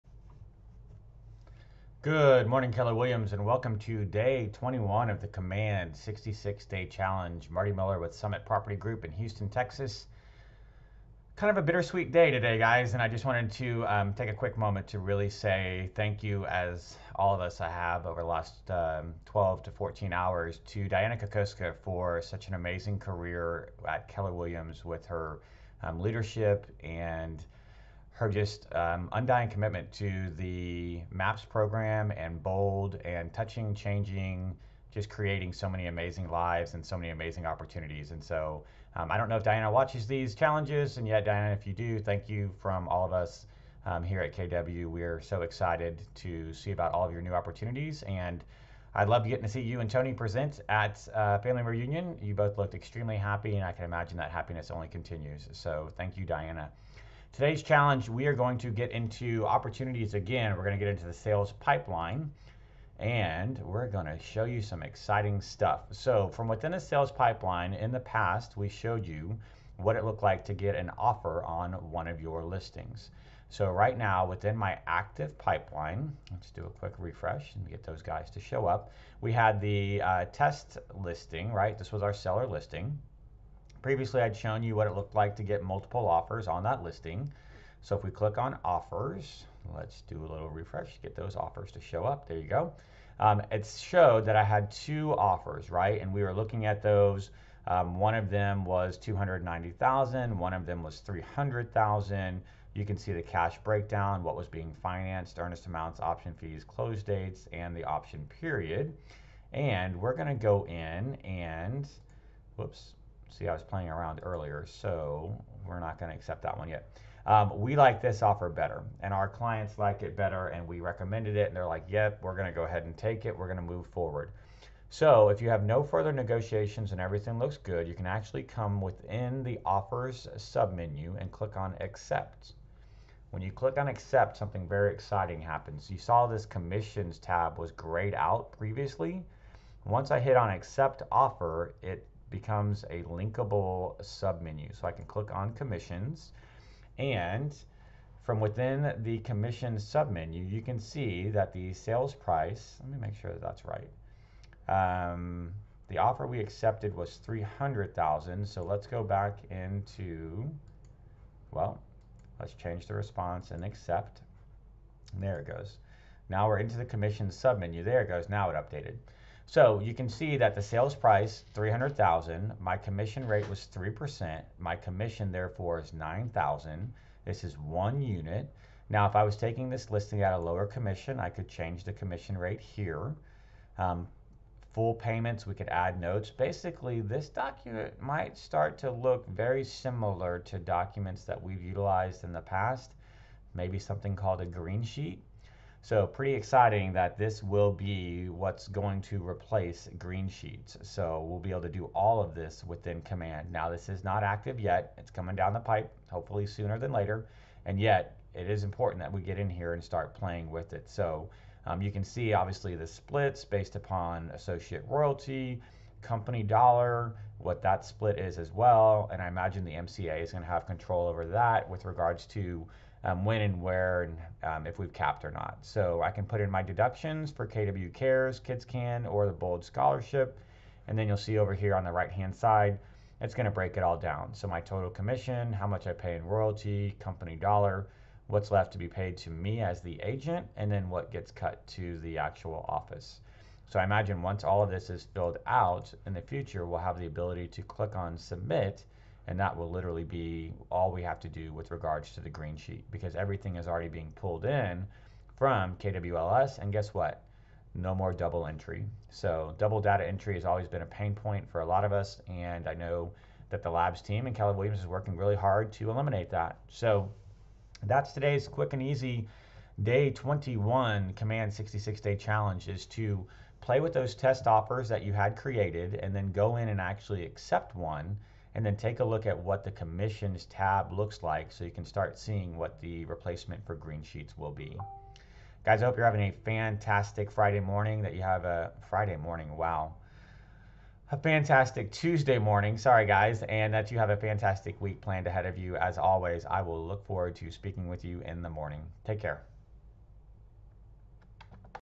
Keller Williams Command 66 Day Challenge 21 - Offers & Commissions
We've been spending time learning KW Command's Opportunities/Sales Pipeline submenu recently.  Previously, we received one or more offers on our listings. We've presented a clear and concise breakdown of those offers and discussed them with our clients. Now we get to accept one and calculate our commissions based upon the accepted offer.

Watch this quick video to learn about to accept an offer and view the Commissions tab which will light up once an offer is accepted. The Commissions tab will be wait for it. the replacement to Greensheets!! This means no more double data entry for your listing details as everything is already saved within the KW Command system! YAY!

Get into one of your listings, review your offers and accept one of them to view the Commissions submenu.